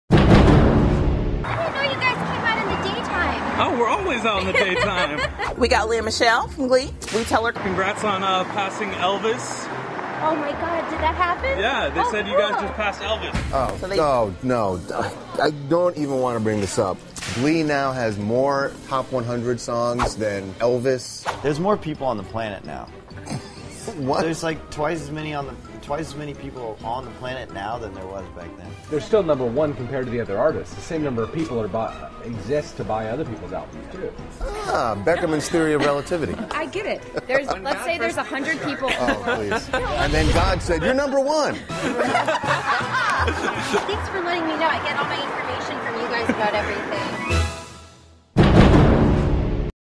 Lea Michele Beats Out Elvis
TMZ breaks the news to Glee cast member Lea Michele that the show has beat out Elvis on the Billboard charts.